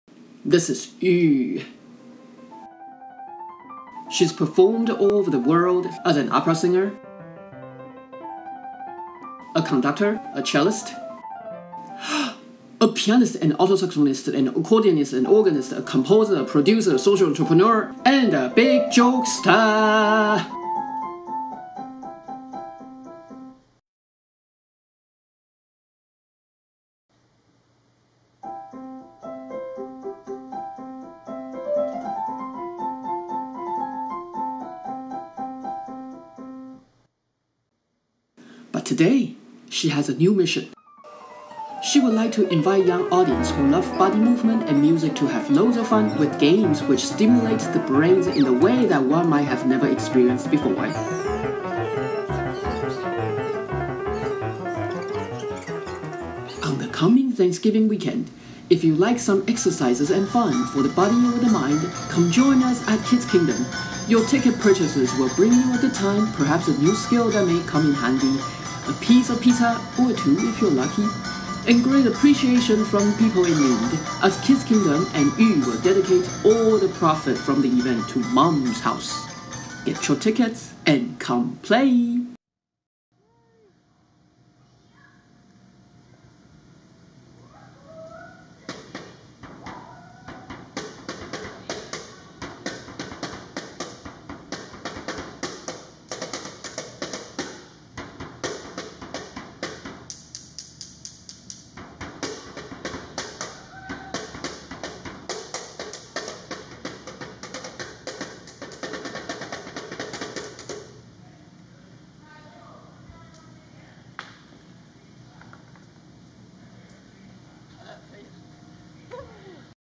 Joy of Playing w/ Ü @ Kidz Kingdom
The super talented and crazy performer, Ü, likes to invite the young audience around Tri Cities Area to have lots of fun at Kidz Kingdom. She'll play games of music, body movement, and story making to burn the calories after Thanksgiving feast and to stimulate the brain cells!!! Your ticket purchases will bring you lots of fun, perhaps a useful skill for the future, a piece of pizza(or two if you're lucky),..., and the great appreciation from kids and parents in need as Ü and Kidz Kingdom will donate all the profit from the event to Mom's House.